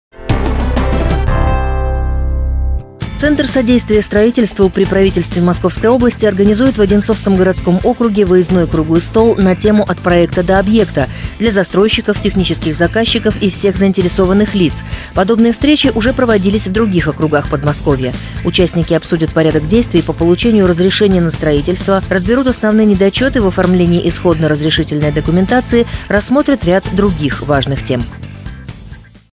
От проекта до объекта
Центр содействия строительству при Правительстве Московской области организует в Одинцовском городском округе выездной круглый стол на тему: «От проекта до объекта» для застройщиков, технических заказчиков и всех заинтересованных лиц.